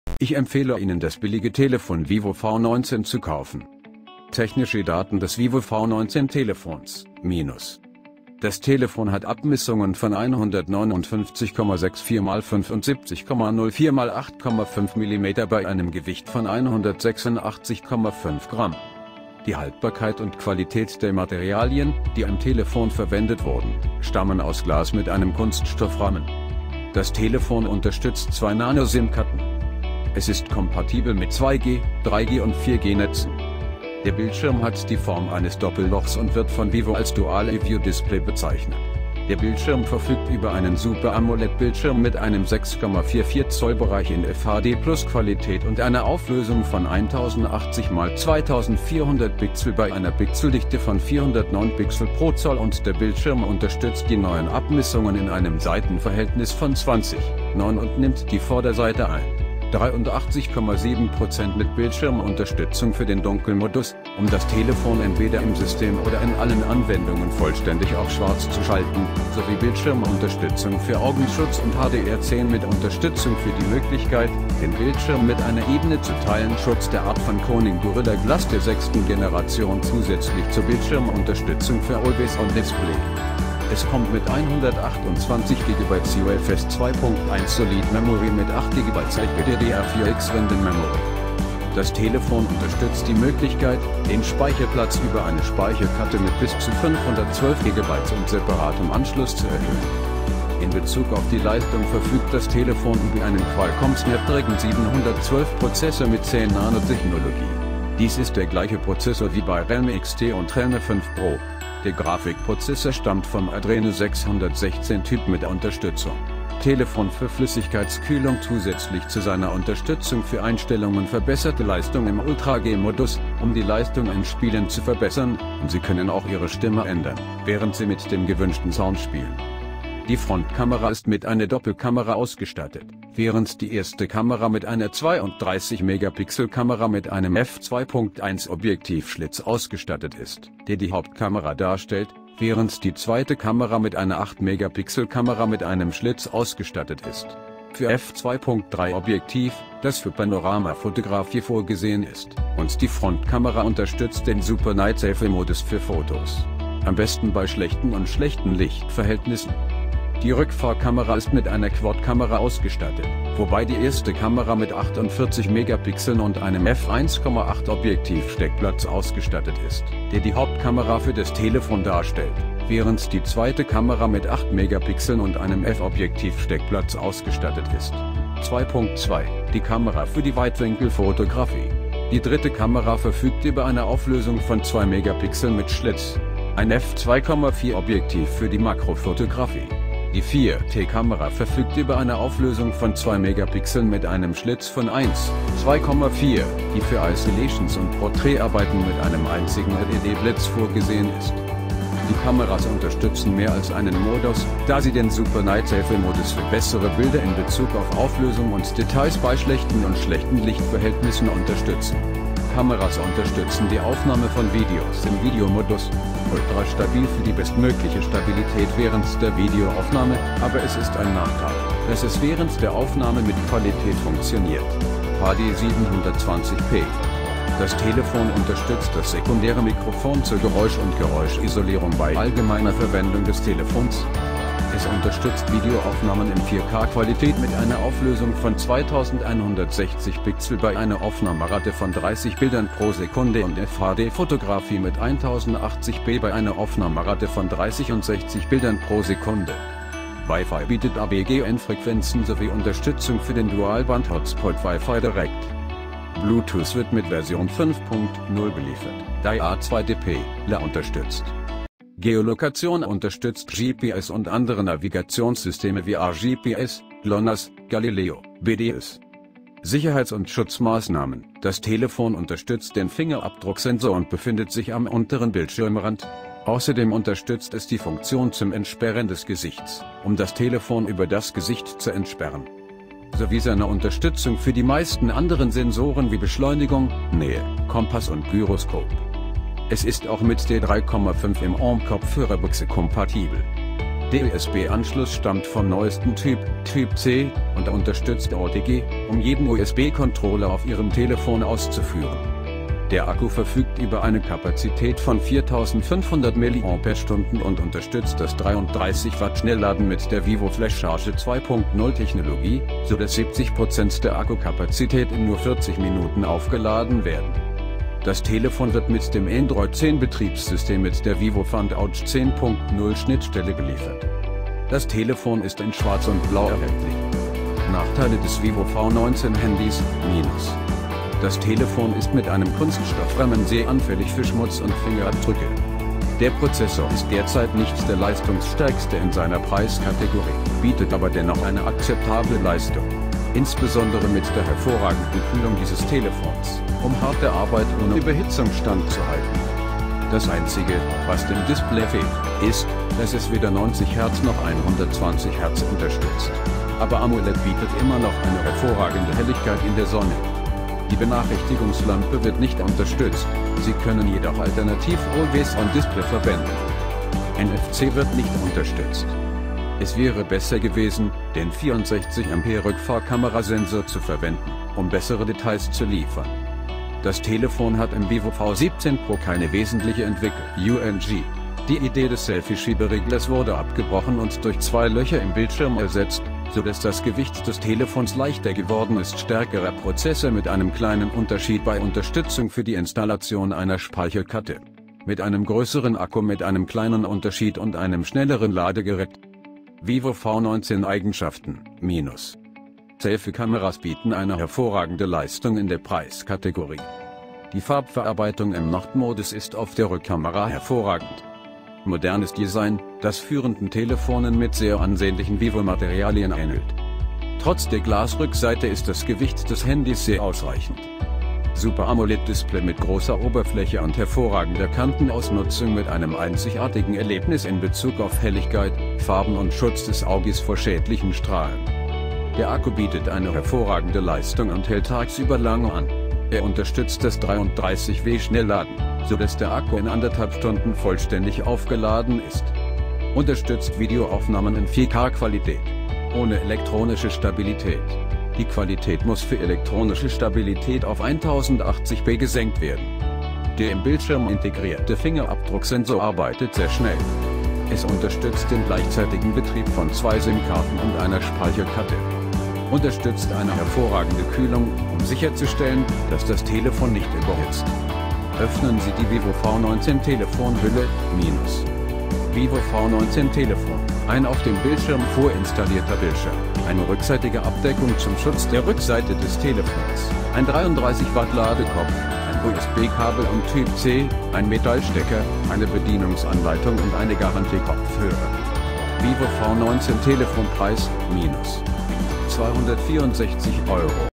Ich empfehle Ihnen das billige Telefon Vivo V19 zu kaufen
Vivo V19
Technische Daten des Vivo V19-Telefons: -
Das Telefon hat Abmessungen von 159,64 x 75,04 x 8,5 mm bei einem Gewicht von 186,5 Gramm.
Die Haltbarkeit und Qualität der Materialien, die im Telefon verwendet wurden, stammen aus Glas mit einem Kunststoffrahmen.
Das Telefon unterstützt zwei Nano Sim-Karten.
Es ist kompatibel mit 2G-, 3G- und 4G-Netzen.
Der Bildschirm hat die Form eines Doppellochs und wird von Vivo als Dual-iView-Display bezeichnet. Der Bildschirm verfügt über einen Super-AMOLED-Bildschirm mit einem 6,44-Zoll-Bereich in FHD + -Qualität und einer Auflösung von 1080 x 2400 Pixel bei einer Pixeldichte von 409 Pixel pro Zoll und der Bildschirm unterstützt die neuen Abmessungen in einem Seitenverhältnis von 20: 9 und nimmt die Vorderseite ein. 83,7% mit Bildschirmunterstützung für den Dunkelmodus, um das Telefon entweder im System oder in allen Anwendungen vollständig auf Schwarz zu schalten, sowie Bildschirmunterstützung für Augenschutz und HDR10 mit Unterstützung für die Möglichkeit, den Bildschirm mit einer Ebene zu teilen Schutz der Art von Corning Gorilla Glass der sechsten Generation Zusätzlich zur Bildschirmunterstützung für Always On Display.
Es kommt mit 128 GB UFS 2.1 Solid Memory mit 8 GB LPDDR4X Random Memory.
- Das Telefon unterstützt die Möglichkeit, den Speicherplatz über eine Speicherkarte mit bis zu 512 GB und separatem Anschluss zu erhöhen.